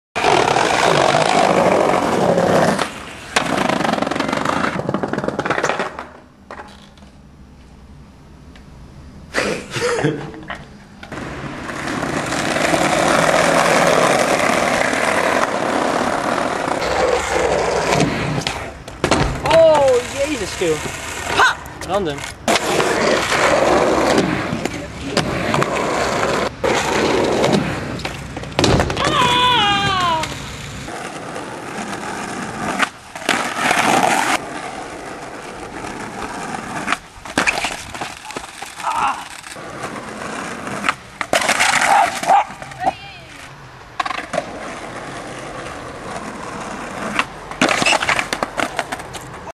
skaterzzz (2006-2007)
Paul Bezuijen, Mitchell Bakker, Chiel Pultrum, Andres Fouché skaten in Hoogezand, Zuidlaren en Groningen.